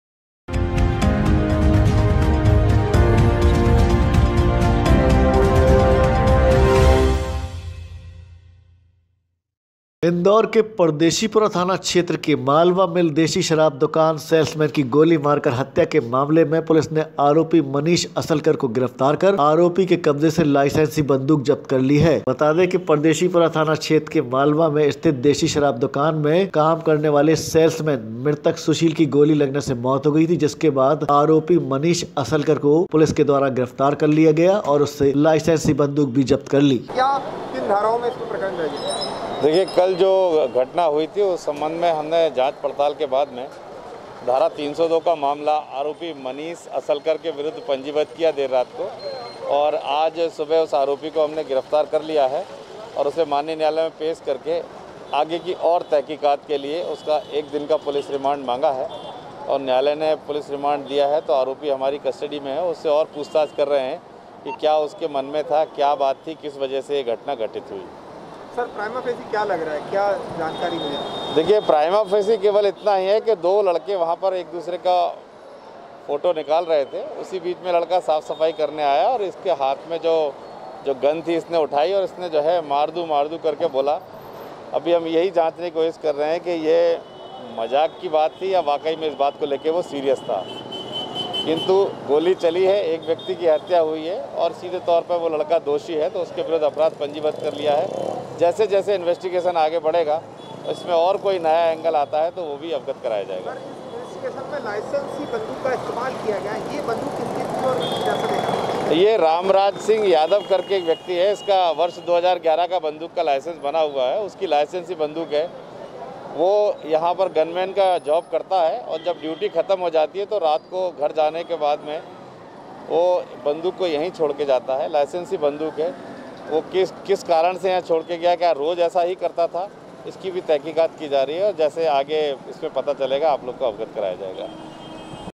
इंदौर : सेल्समैन की गोली मारकर हत्या के मामले में पुलिस ने आरोपी को गिरफ्तार कर आरोपी से
कि सुशील और उसका एक अन्य साथी बंदूक के साथ फोटो खिंचवा रहा था तभी दुकान में साफ सफाई करने वाला कर्मचारी मनीष असलकर साफ सफाई के दौरान उसे 12 बोर की बंदूक उठा ली और फोटो खिंचवाने के लिए बंदूक उठाई और मार दूंगा मार दूंगा बोलते बोलते मजाक मस्ती में आखिरकार बंदूक का ट्रिगर दब गया और गोली चल गई जो गोली सुशील के सीने के आर पार हो गई घटना पर ही 20 वर्षीय सुशील की मौके पर मौत हो गई थी इस घटना के बाद पुलिस ने हत्या का प्रकरण दर्ज कर आरोपी मनीष असलकर को गिरफ्तार कर लिया है